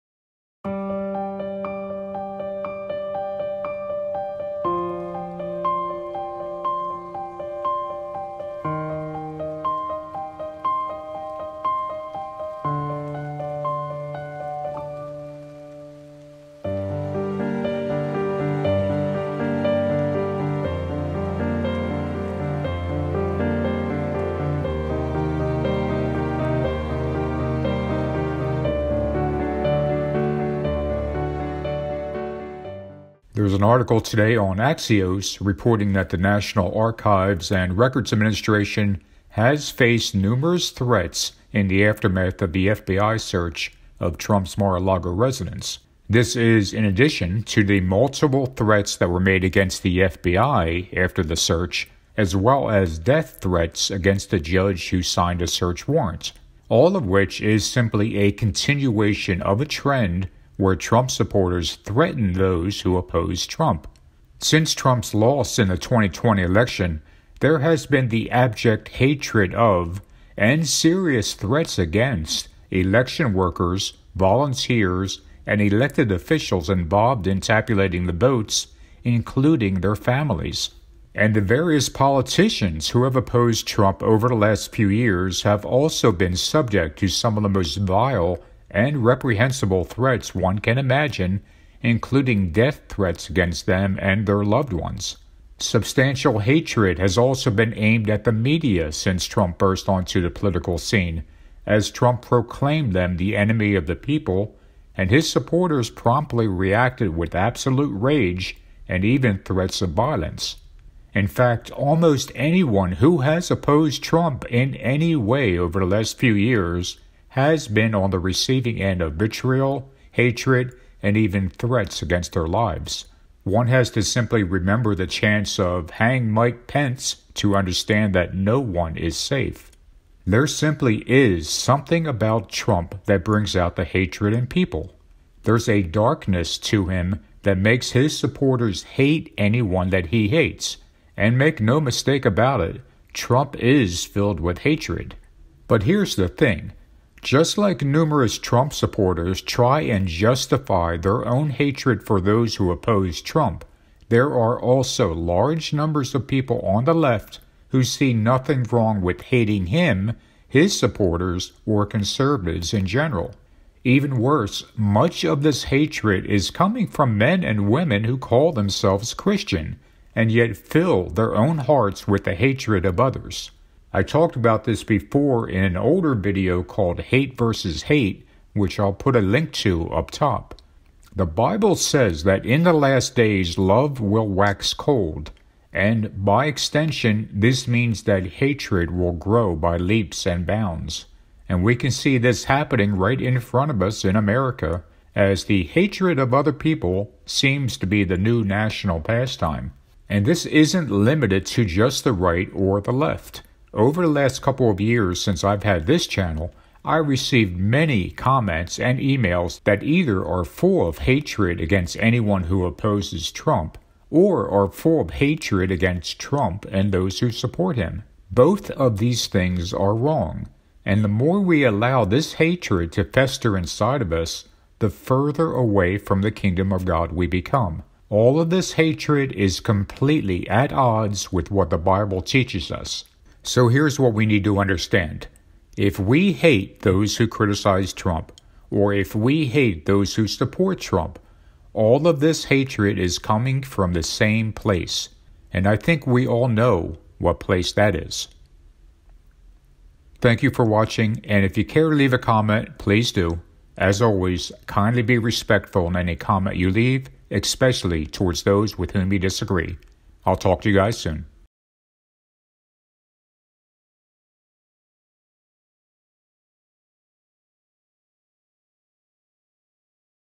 Trump Brings Out the Hatred in All of Us
Trump has the innate ability to bring out the hatred from within us, no matter our political beliefs.

There simply IS something about Trump that brings out the hatred in people. There's a darkness to him that makes his supporters hate anyone that he hates – and make no mistake about it, Trump is filled with hatred.

CHAPTERS
0:00 Intro music
0:33 Threats against the National Archives
1:04 Trump's enemies are always threatened
2:10 There's something about Trump
2:43 The hatred is coming from Christians
4:09 Support channel

If you enjoyed this video, please remember to click on the "like" button as that greatly helps out my channel with YouTube's algorithms.

VIDEO TRANSCRIPT
There's an article today on Axios reporting that the National Archives and Records Administration has faced numerous threats in the aftermath of the FBI search of Trump's Mar-a-Lago residence.

This is in addition to the multiple threats that were made against the FBI after the search, as well as death threats against the judge who signed the search warrant. All of which is simply a continuation of a trend where Trump supporters threaten those who oppose Trump.

Since Trump's loss in the 2020 election there has been the abject hatred of – and serious threats against - election workers, volunteers, and elected officials involved in tabulating the votes, including their families.

And the various politicians who have opposed Trump over the last few years have also been subject to some of the most vile and reprehensible threats one can imagine, including death threats against them and their loved ones.

Substantial hatred has also been aimed at the media since Trump burst onto the political scene, as Trump proclaimed them the enemy of the people and his supporters promptly reacted with absolute rage and even threats of violence.

In fact, almost anyone who has opposed Trump in any way over the last few years has been on the receiving end of vitriol, hatred, and even threats against their lives. One has to simply remember the chants of “hang Mike Pence” to understand that no one is safe.

But here's the thing: just like numerous Trump supporters try and justify their own hatred for those who oppose Trump, there are also large numbers of people on the left who see nothing wrong with hating him, his supporters, or conservatives in general.

Even worse, much of this hatred is coming from men and women who call themselves Christian, and yet fill their own hearts with the hatred of others. I talked about this before in an older video called Hate vs. Hate, which I'll put a link to up top.

The bible says that in the last days love will wax cold, and by extension, this means that hatred will grow by leaps and bounds. And we can see this happening right in front of us in America, as the hatred of other people seems to be the new national pastime.

And this isn't limited to just the right or the left. Over the last couple of years since I've had this channel I've received many comments and emails that either are full of hatred against anyone who opposes Trump, or are full of hatred against Trump and those who support him.

BOTH of these things are wrong, and the more we allow this hatred to fester inside of us, the further away from the Kingdom of God we become. All of this hatred is completely at odds with what the bible teaches us.

So here's what we need to understand: if we hate those who criticize Trump, or if we hate those who support Trump, all of this hatred is coming from the same place. And I think we all know what place that is.

As always, kindly be respectful in any comment you leave, especially toward those with whom you disagree. I'll talk to you guys soon. ♦